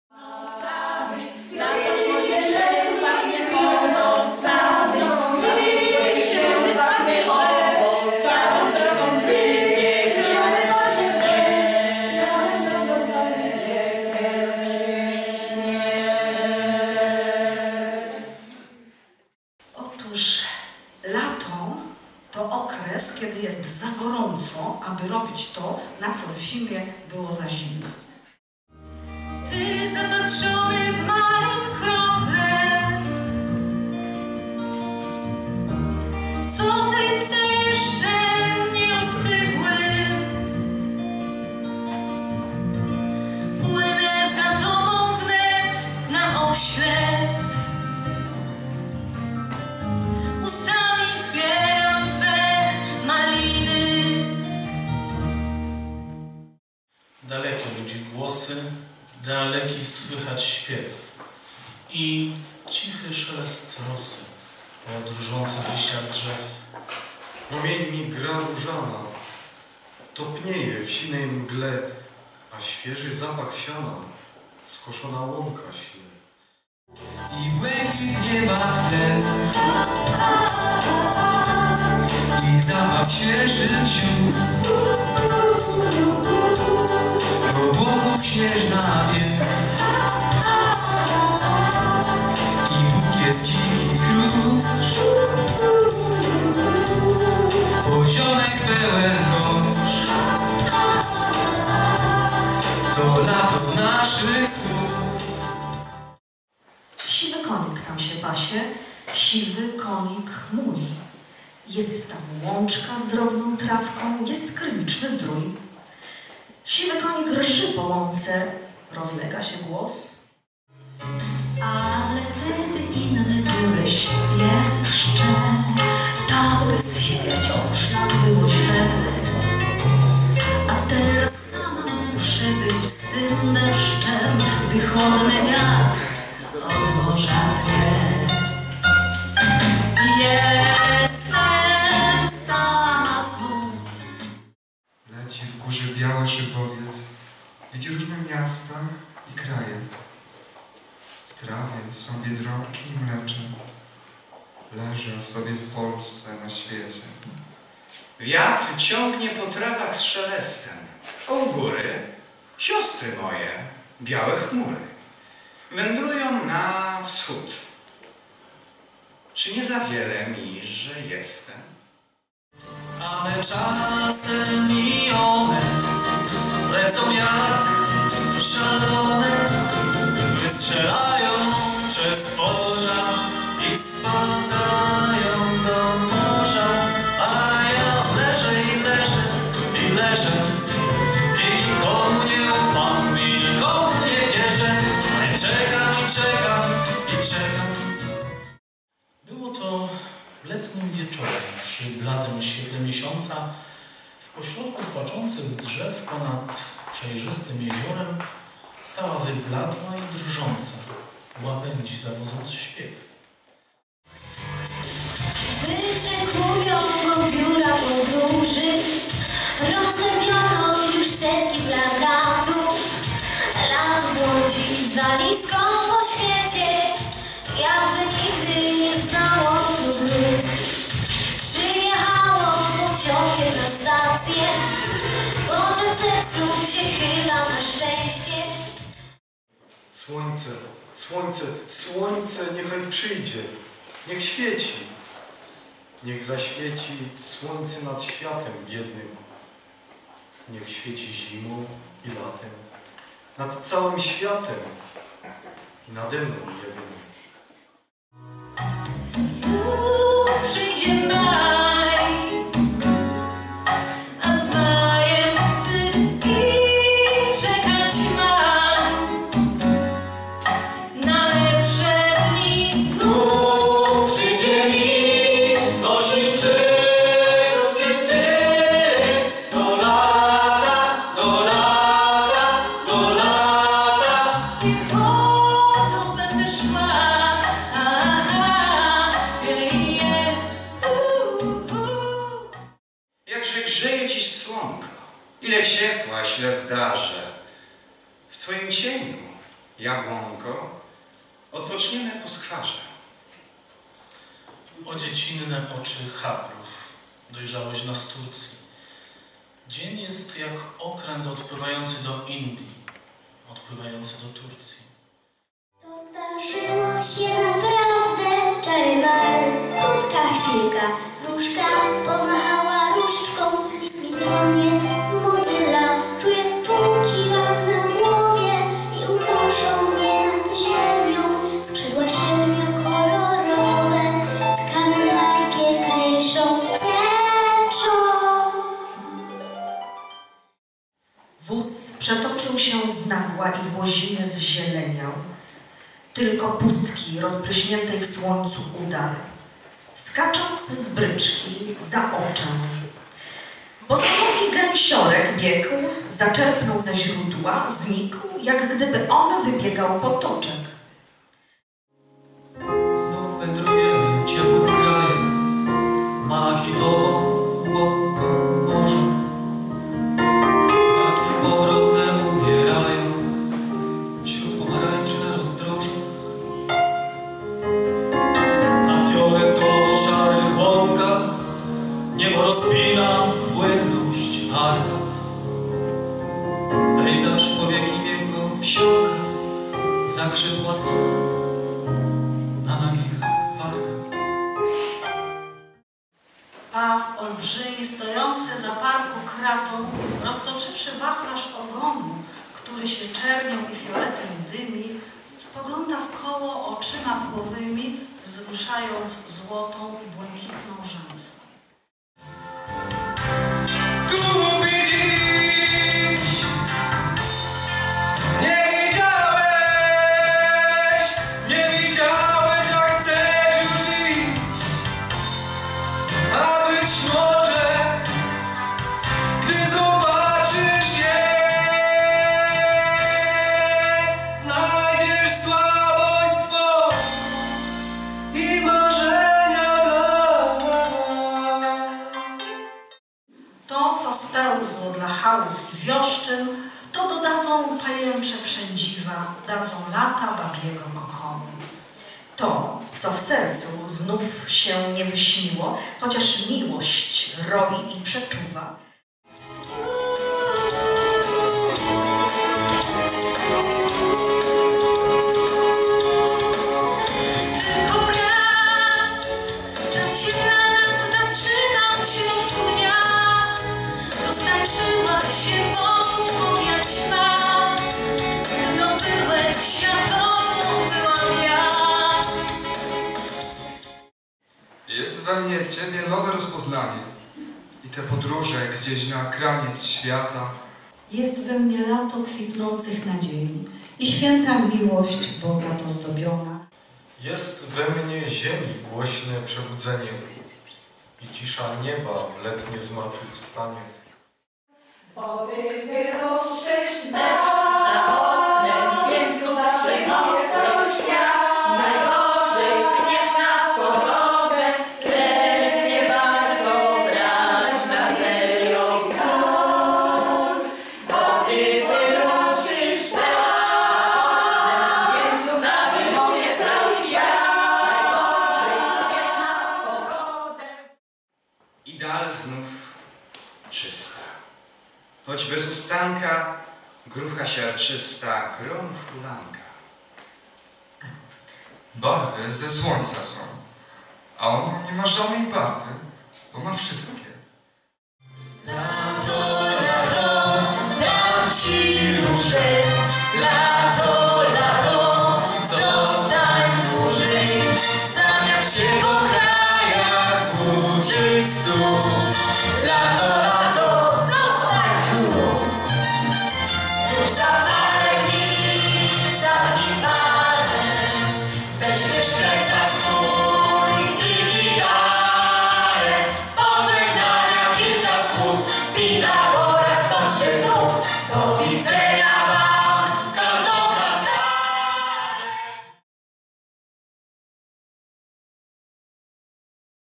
Śpiewali i recytowali o lecie w "13"
Ostatnie w tym sezonie spotkanie z cyklu "Poezja i muzyka pod 13" upłynęło pod znakiem lata. Scenografię stanowiły obrazy przedstawiające tę porę roku i stanowiły idealne tło do wykonywanych tekstów. Goście mogli podziwiać między innymi przepiękne prace plastyczne pani Moniki Balcer-Konieczny oraz pani Anny Stoszek-Szrobosz.